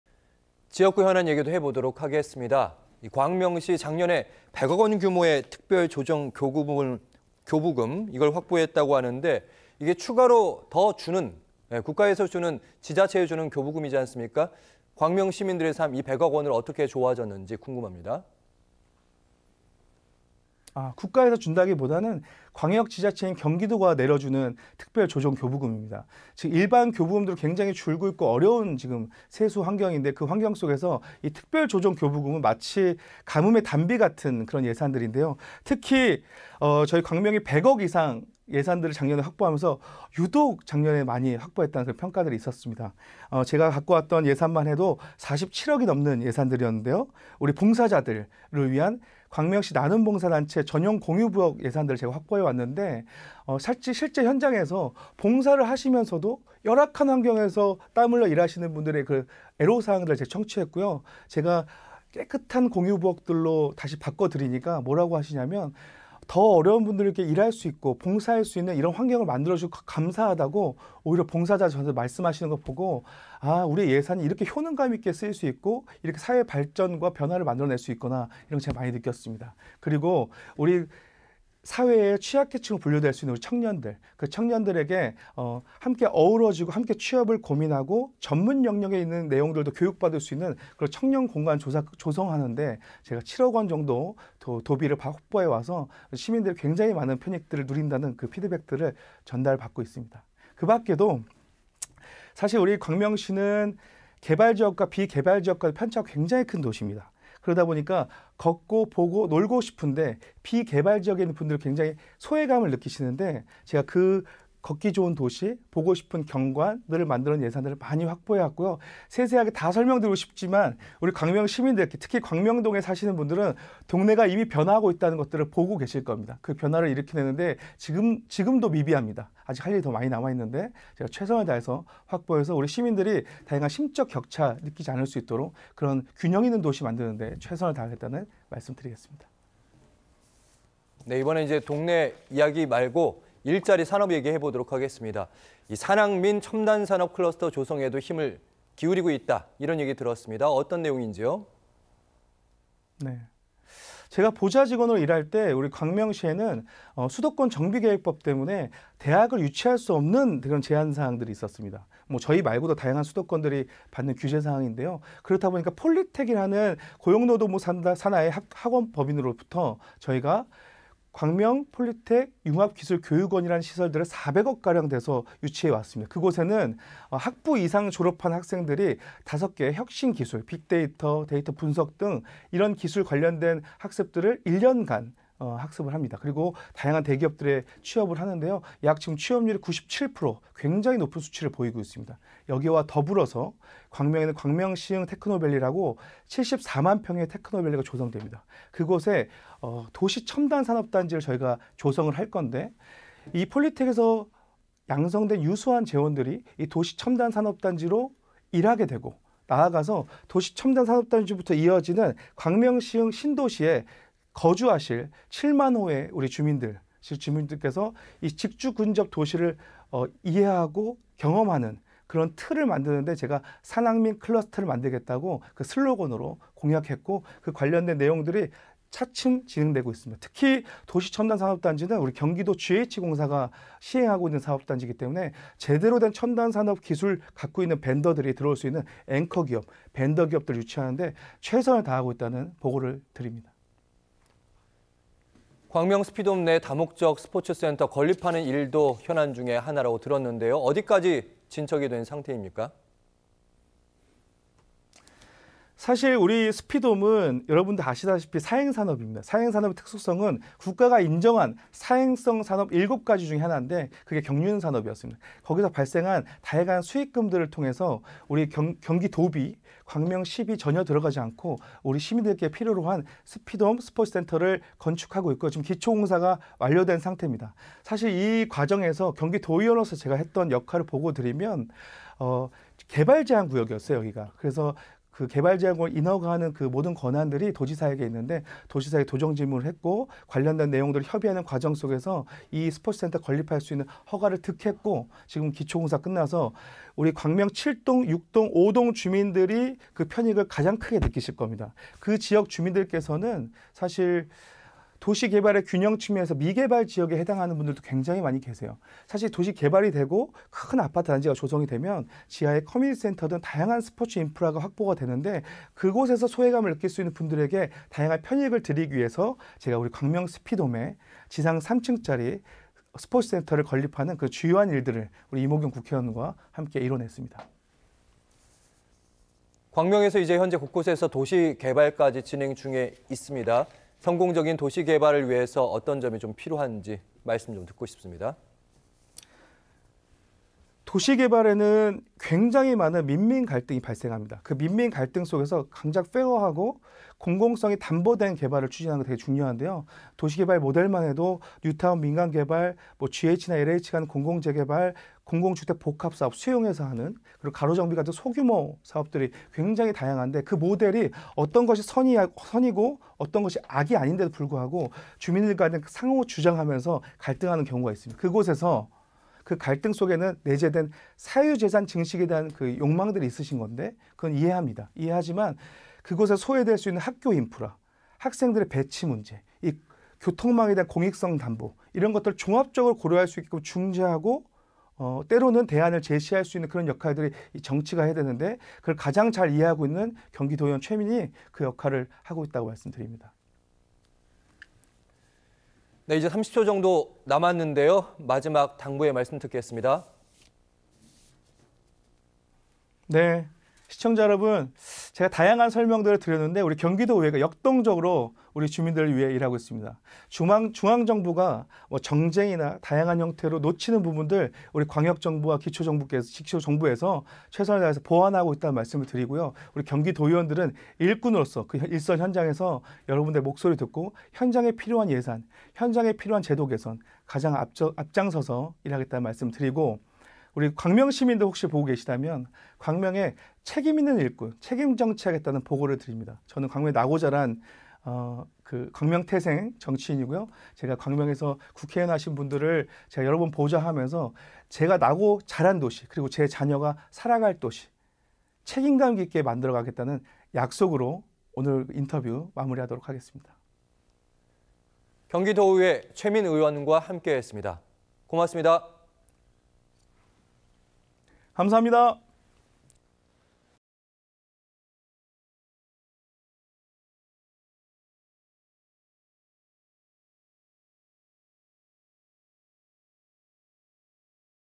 청년 세대를 대변하는 정치인... "100억 교부금이 바꾼 광명 발전"
지역구 현안 얘기도 해보겠습니다.
광명시는 작년에 100억 원 규모의
특별조정교부금을 확보하기도 했는데요.
특별조정교부금으로 광명시민들 삶이
더 좋아지고 있는지 그리고 광명 스피돔에 내 다목적
스포츠 센터를 건립하는 일도 중요한 현안 중 하나입니다.
경기도의회 여성가족평생교육위원회 소속 최민 의원에게
더 자세하게 물어보았습니다.